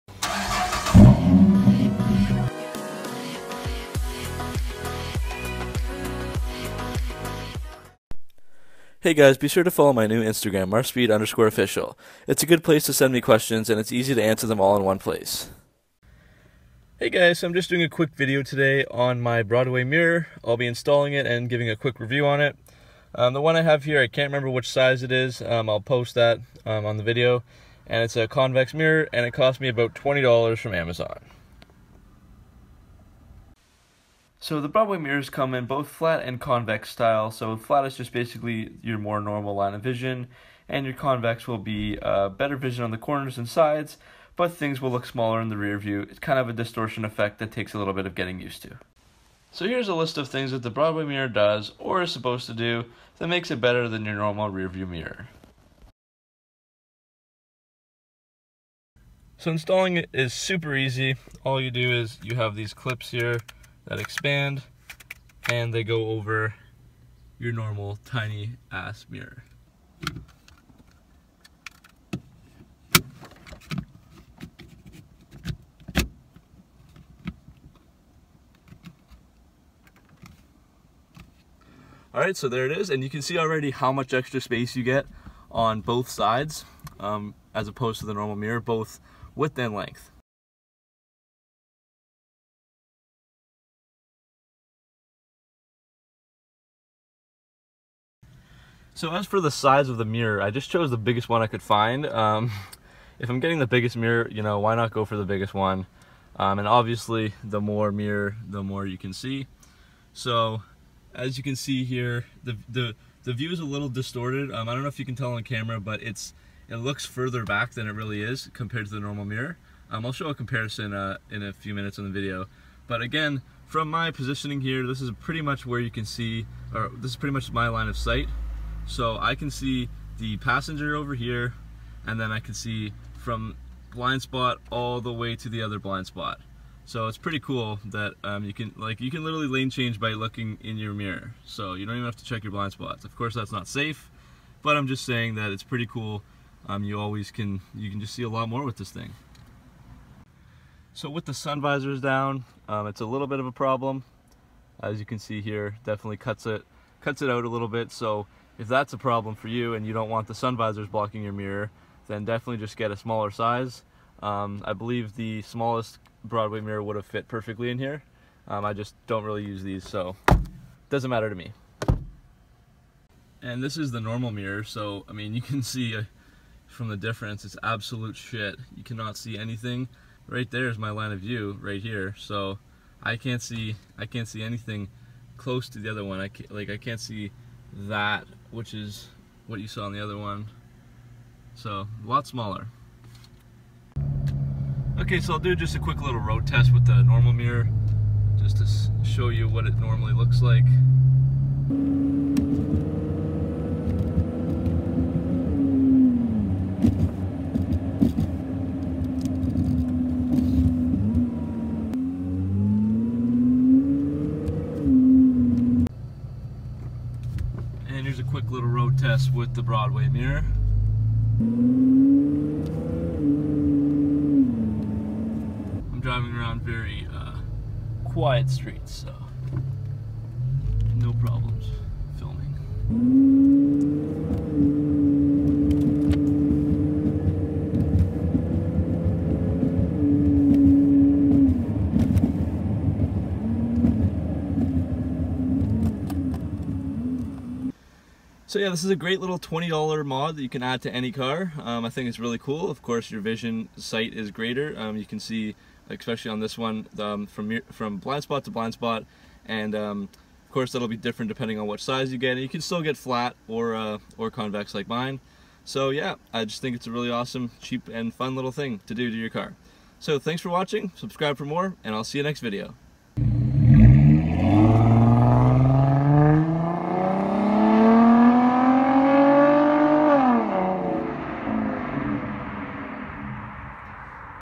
Broadway Mirror 400 mm Install & Review -- COOL CHEAP MOD!
Quick video showing the installation, review, and usage of a 400 mm Broadway wide angle rearview mirror! This is a great cheap little interior mod you can do to any car, and it not only looks cool but gives you a much larger line of vision and reduces the blindspots!

400 mm Broadway Mirror Convex:

400 mm Broadway Mirror Flat:

Car Radios & Stereos:
Save 5% on any Seicane order with the code "MARSPEED"